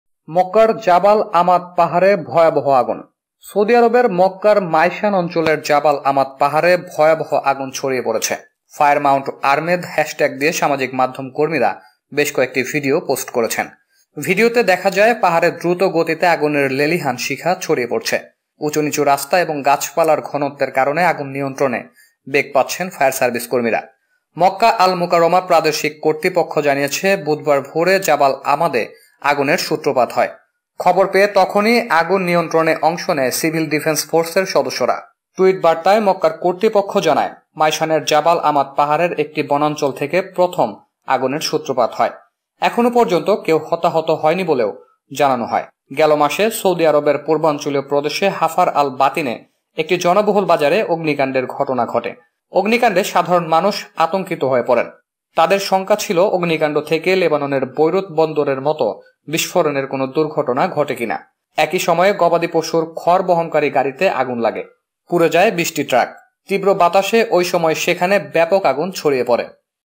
সৌদি আরবে মক্কার জাবাল আমাদ পাহাড়ে ভয়াবহ আগুন। আতঙ্কিত হাজারো মানুষ।
IDesk#মক্কা

সৌদি আরবের মক্কার মায়সান অঞ্চলের জাবাল আমাদ পাহাড়ে ভয়াবহ আগুন ছড়িয়ে পড়েছে।ফায়ার মাউন্ট আরমেদ হ্যাশট্যাগ দিয়ে সামাজিক মাধ্যমকর্মীরা বেশ কয়েকটি ভিডিও পোস্ট করেছেন। ভিডিওতে দেখা যায়, পাহাড়ে দ্রুতগতিতে আগুনের লেলিহান শিখা ছড়িয়ে পড়ছে। উচুনিচু রাস্তা এবং গাছপালার ঘনত্বের কারণে আগুন নিয়ন্ত্রেণে বেগ পাচ্ছেন ফায়ার সার্ভিসকর্মীরা।